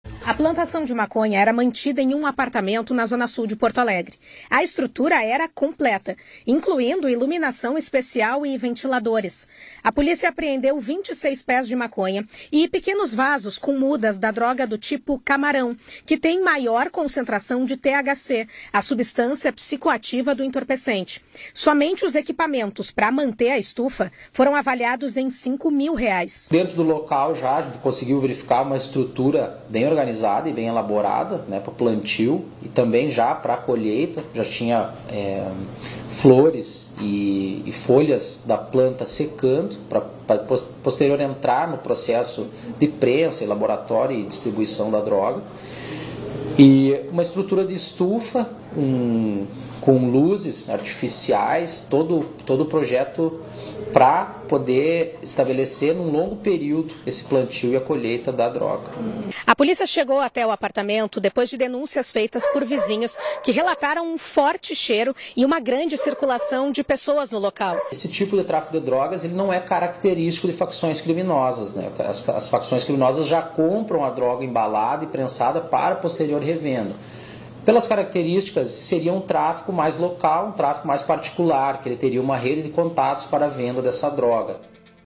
Zona sul da capital: Polícia fecha estufa de cultivo de maconha
Na zona sul da capital, a polícia encontrou uma estufa de maconha. Os equipamentos apreendidos no locam foram avaliados em cinco mil reais.